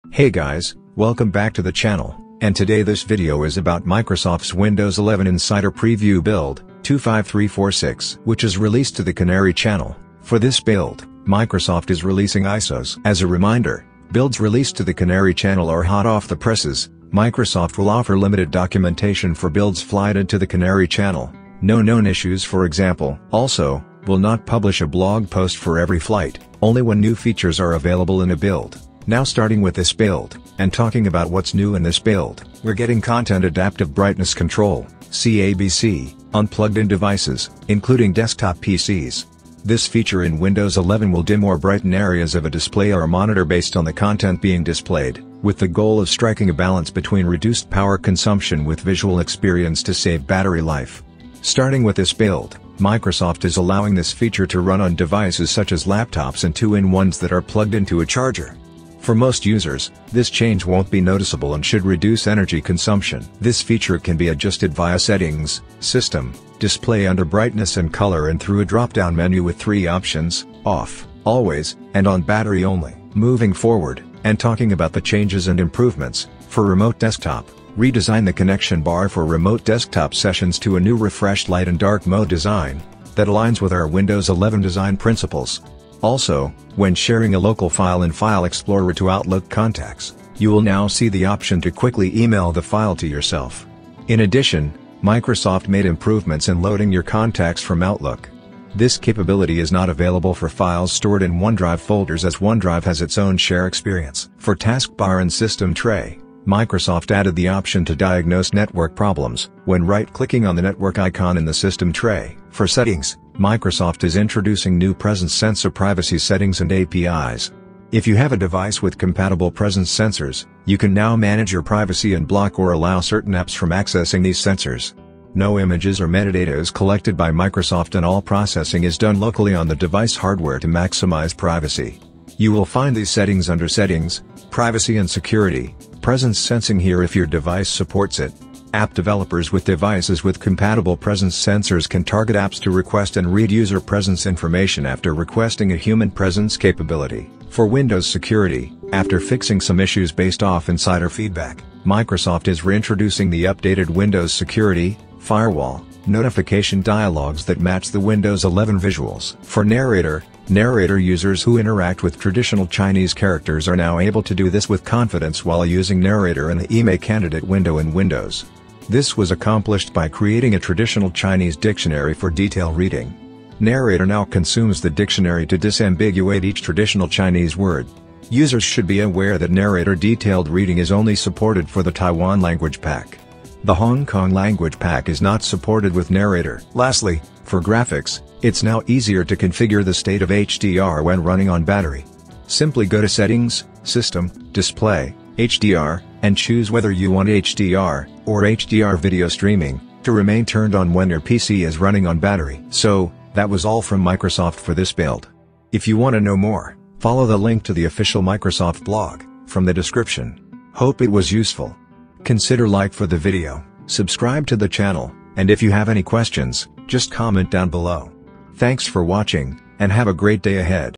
Windows11 Insider Preview Build 25346.1001: ISOs for Build, New Settings, Changes & Improvements
CHAPTERS

GET SUPPORT
► Support:
► Forum: